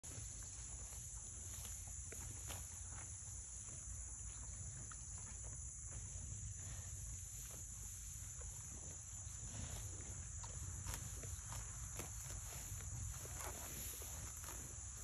Angus / limousin bulls chewing on fresh hay. Moo.
Limousin and angus breed cattle.